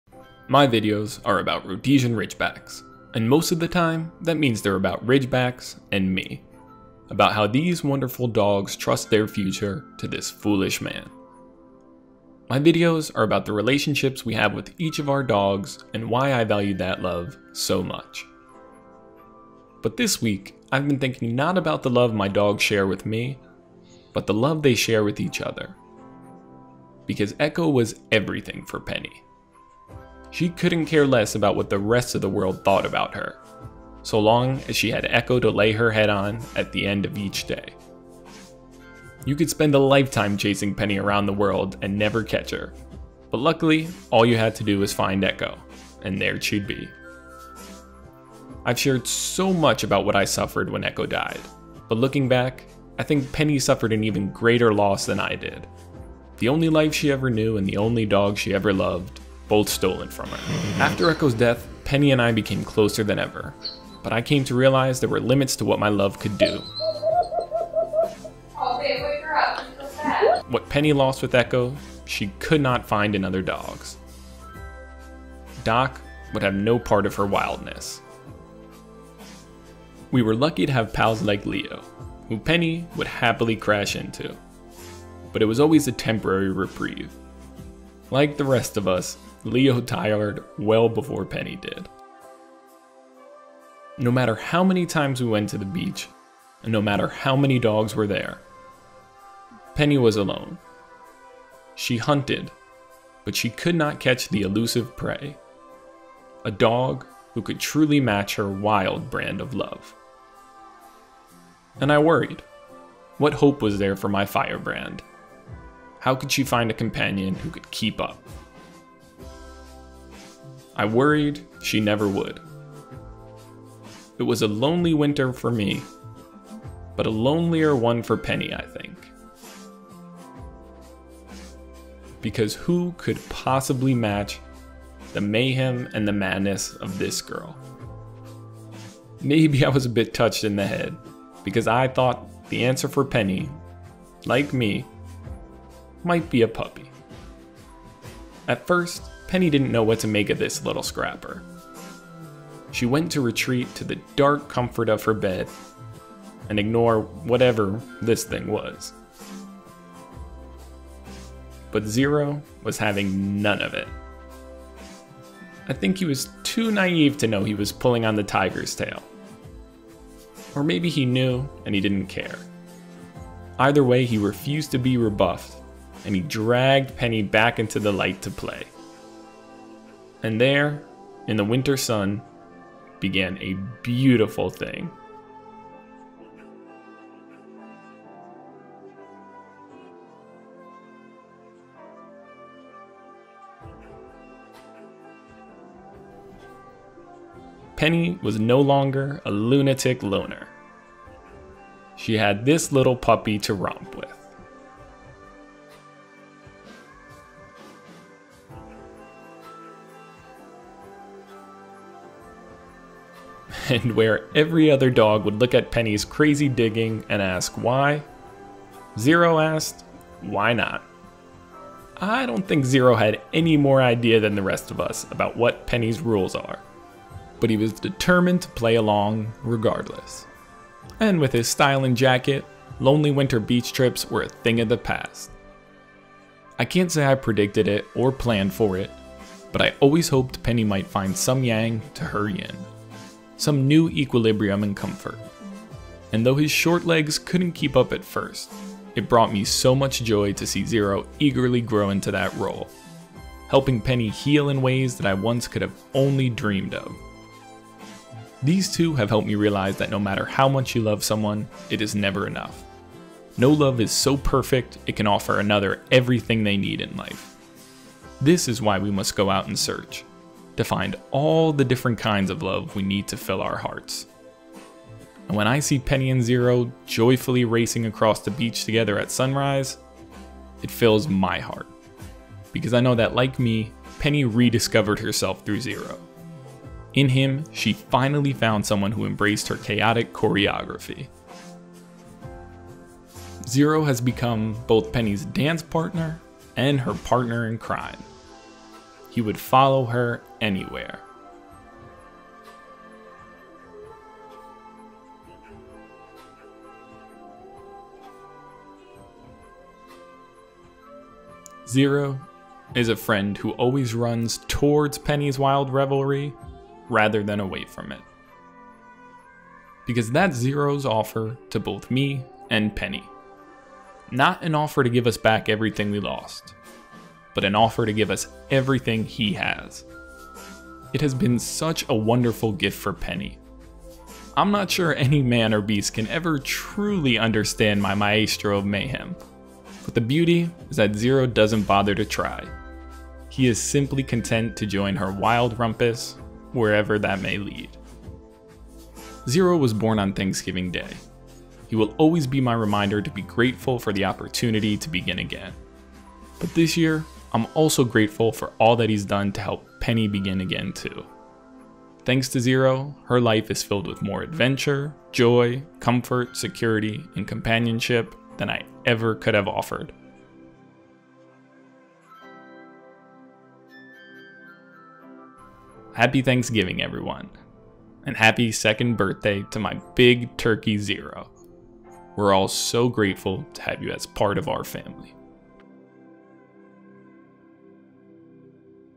Thankful My Rhodesian Ridgebacks Have Each Other
I often talk about how thankful I am to have my dogs, but I'm also grateful they have each other. With Zero turning two this weekend, I took a look back at how he's helped Penny rediscover herself. It means the world to me that these two have grown so close and are always happiest together.  Happy Thanksgiving and happy birthday Zero!